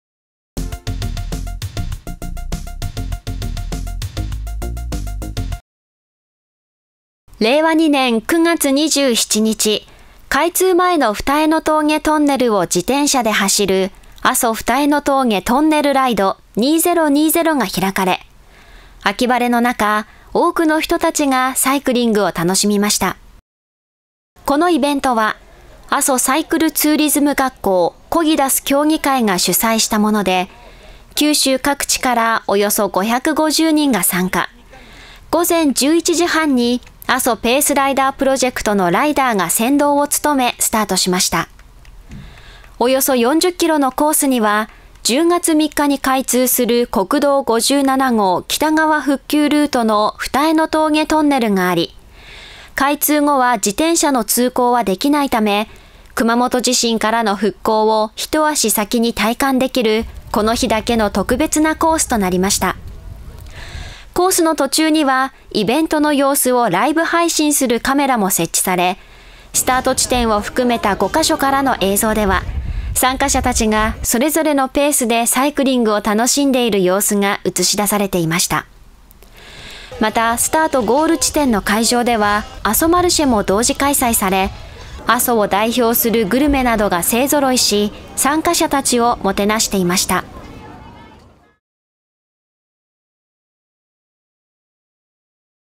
ASO二重峠トンネルライド2020
令和2年9月27日、開通前の二重峠トンネルを自転車で走る「ASO二重峠トンネルライド2020」が開かれ、秋晴れの中多くの人たちがサイクリングを楽しみました。